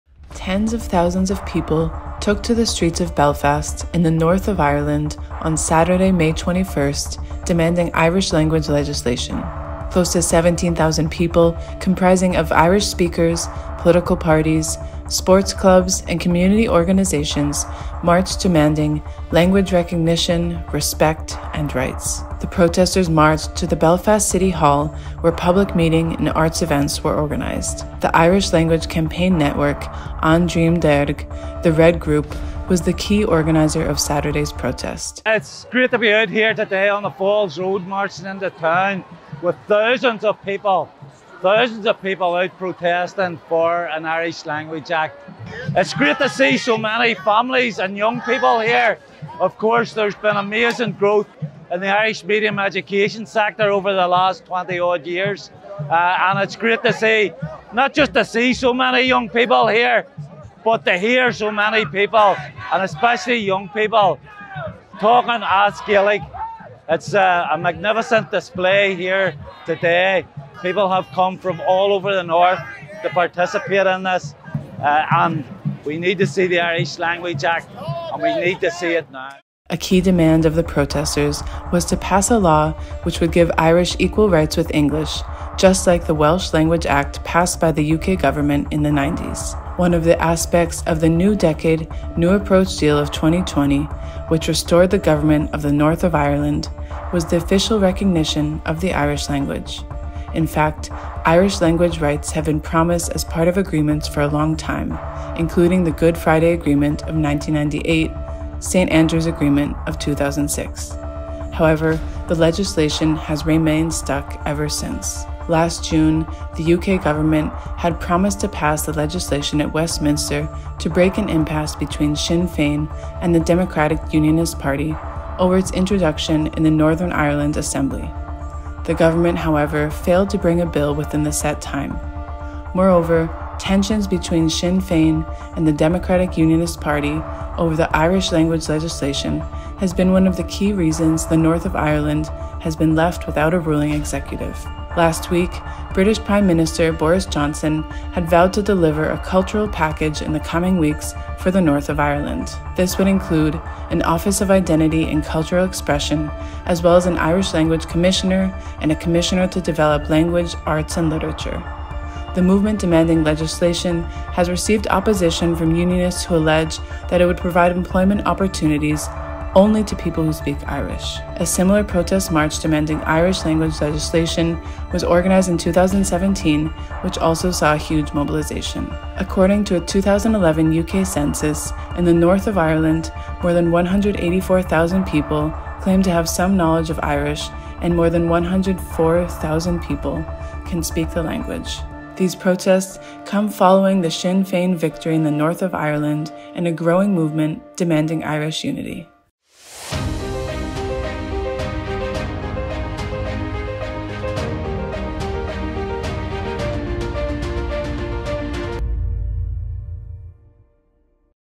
Tens of thousands rally in North of Ireland for Irish language legislation
Tens of thousands of people took to the streets of Belfast, in the North of Ireland on Saturday, May 21. The central demand of the protesters was to pass a law which would give Irish equal rights with English similar to the Welsh Language Act passed by the UK Government in the 90s.